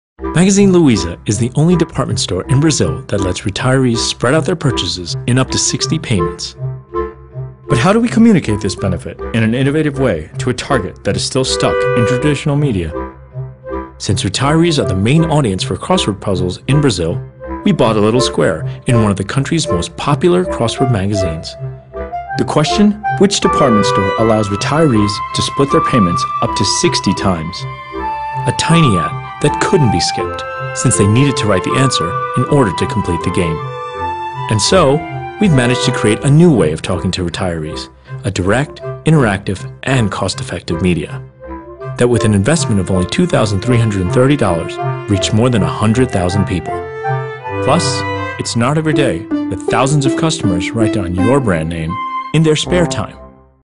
Magazine Luiza - The Crossword AD
Through Crossword, one of the favorite games of retirees, we could tell this audience the advantages of buying Magazine Luiza. An innovative approach, low cost and wide reach.